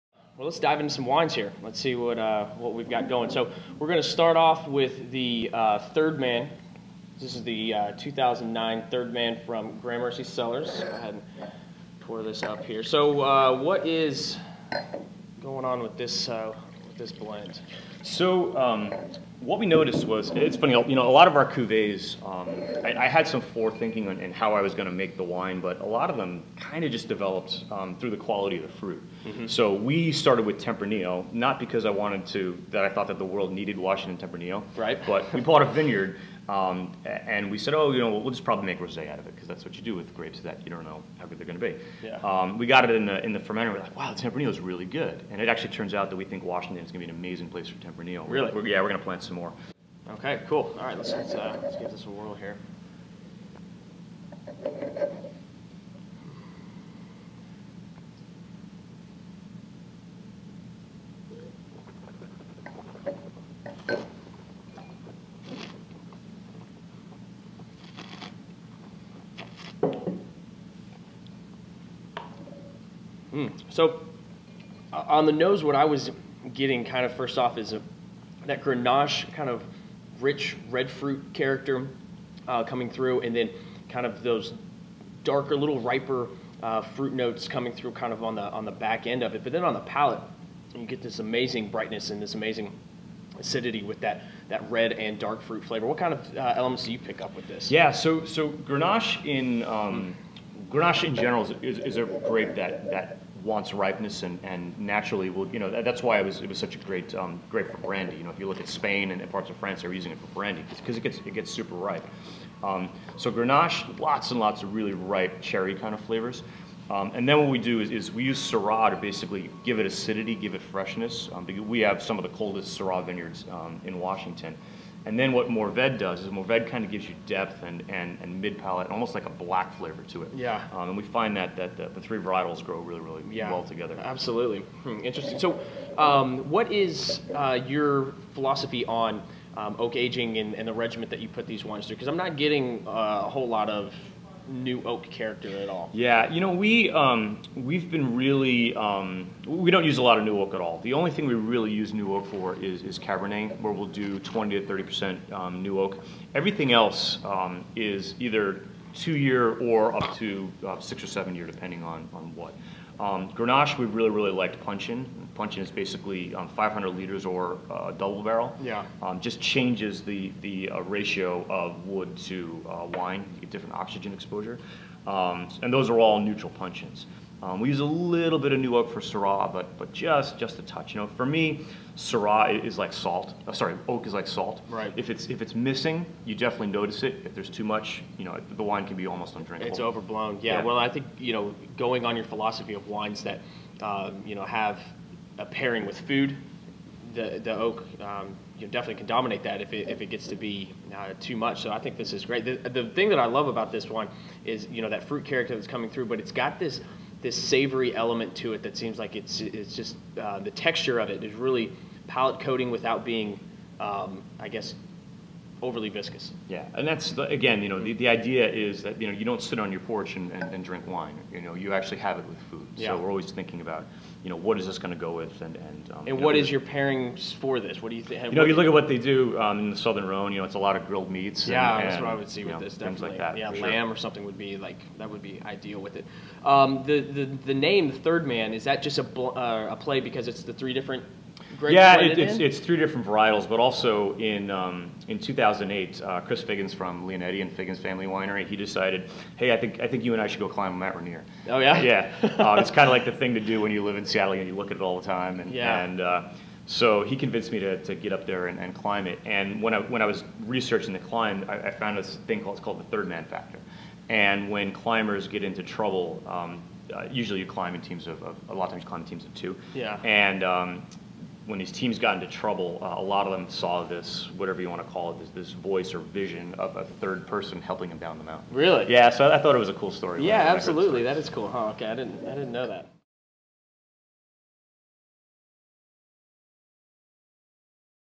Greg Harrington & Gramercy Cellars (2/4) - w/ Bill Elsey for Wines.com TV
Wines.com Sommelier Bill Elsey interviews Master Sommelier Greg Harrington.

& Follow on Twitter @WinesCom
and Check out for great deals on wine & accessories!!